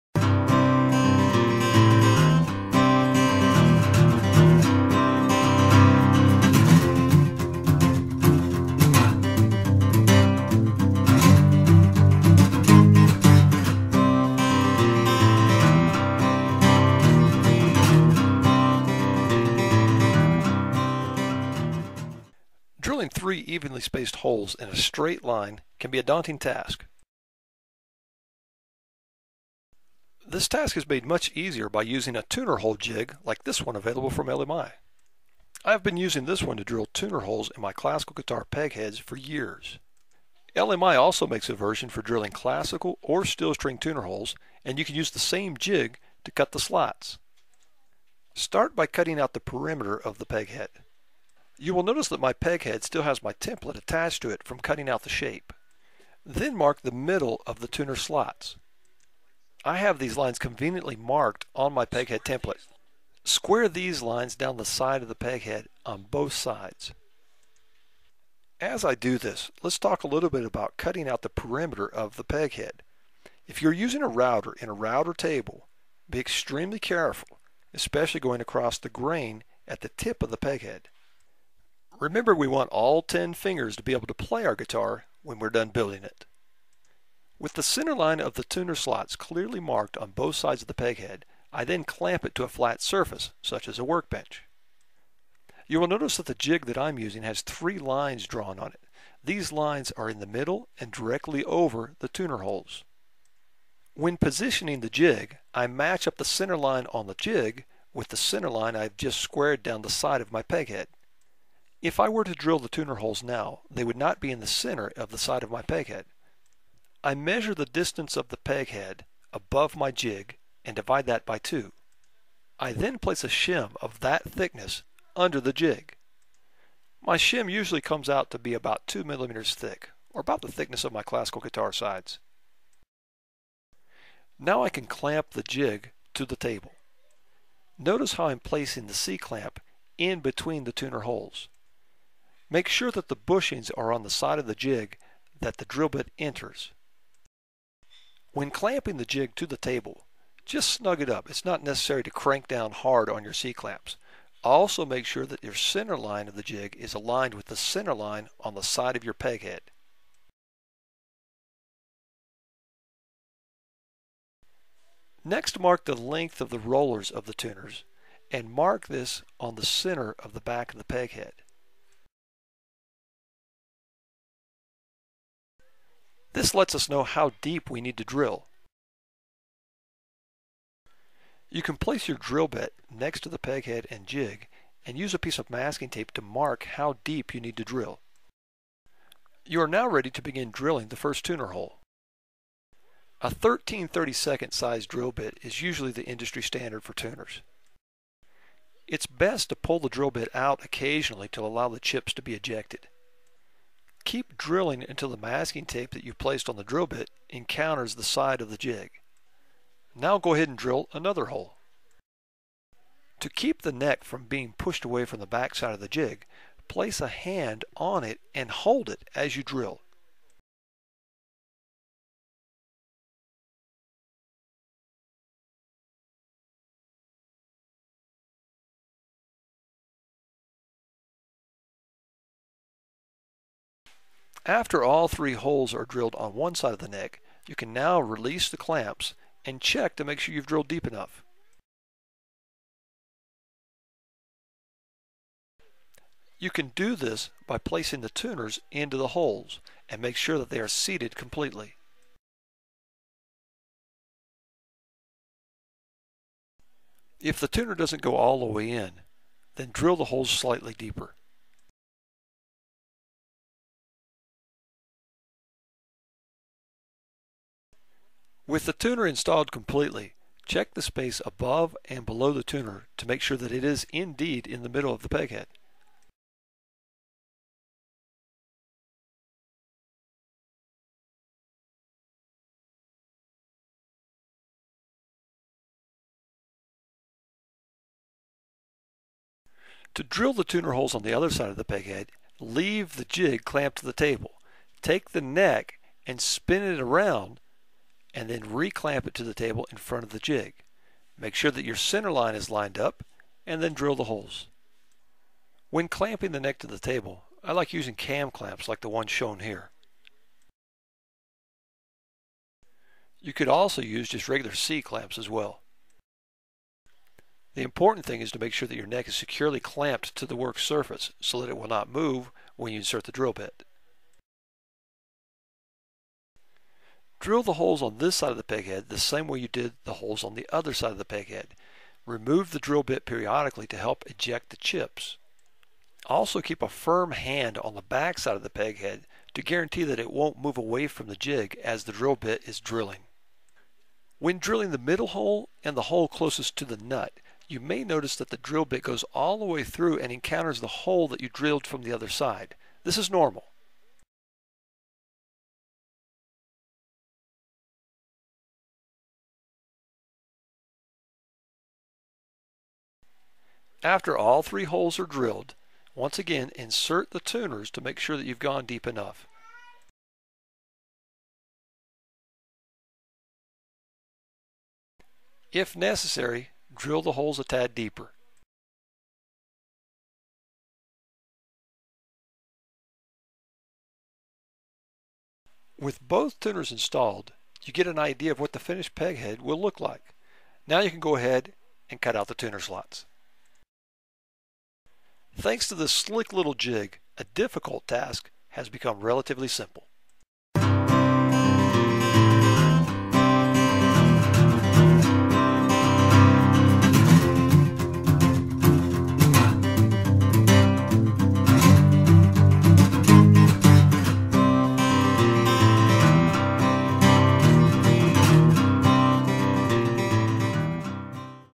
O'Brien Guitars - Luthier Tips du Jour- Drilling Tuner Holes
Luthier and Instructor Robert O'Brien shows how to drill tuner holes